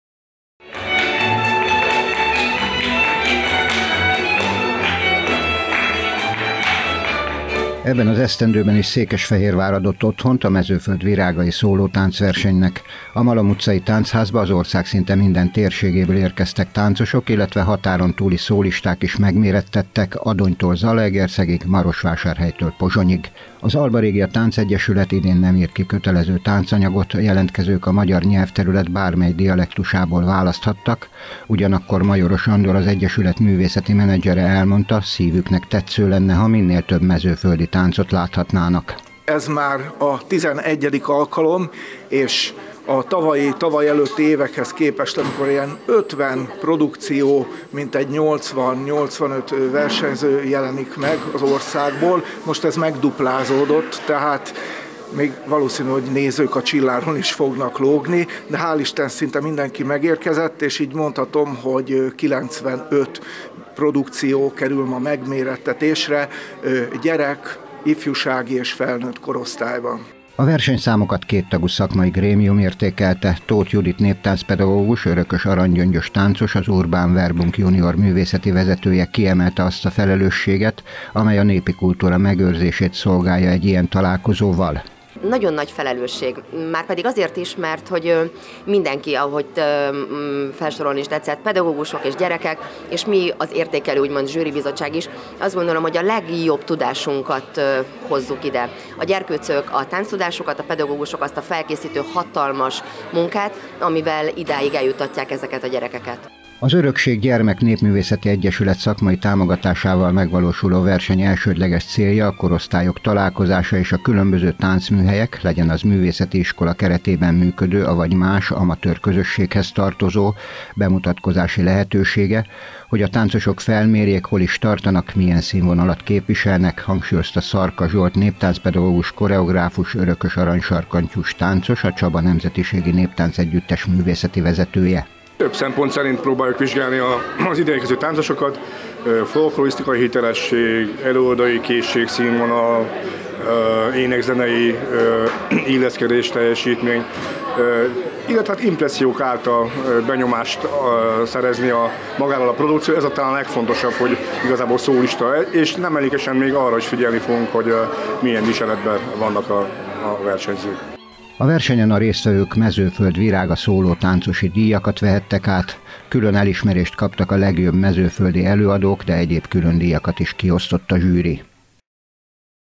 Mezőföld virágai
11. alkalommal rendezte meg a Mezőföld Virágai szólótánc versenyt az Alba Regia Táncegyesület. Idén négy korosztályban 95 táncos mutatta meg elkötelezettségét a népi kultúra iránt.